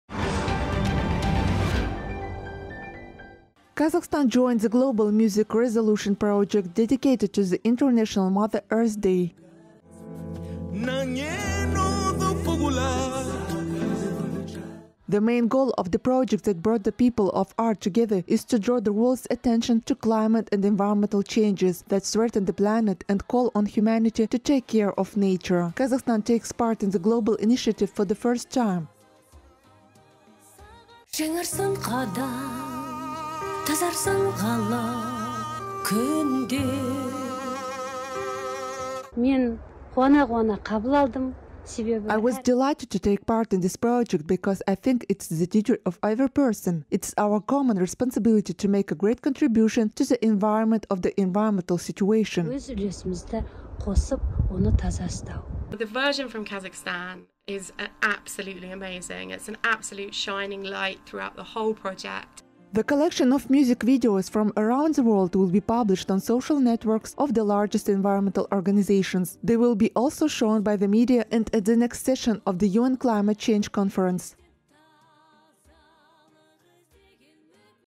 Kazakhstan joined the global music Resolution project. Qazaq TV
Kazakhstan joined the global music Resolution project dedicated to the International Mother Earth Day.

The main goal of the project that brought the people of art together is to draw the world’s attention to climate and environmental changes that threaten the planet and call on humanity to take care of nature. Kazakhstan takes part in the global initiative for the first time.

“I was delighted to take part in this project, because I think that it is the duty of every person. It is our common responsibility to make a great contribution to the improvement of the environmental situation,” said Tamara Assar, a singer, the Honored Figure of Kazakhstan.

“The version from Kazakhstan is absolutely amazing. It’s an absolute shining light throughout the whole project,” said Abbey Wright, Director of Resolution Project.

The collection of music videos from around the world will be published on social networks of the largest environmental organizations. They will be also shown by the media and at the next session of the UN Climate Change conference.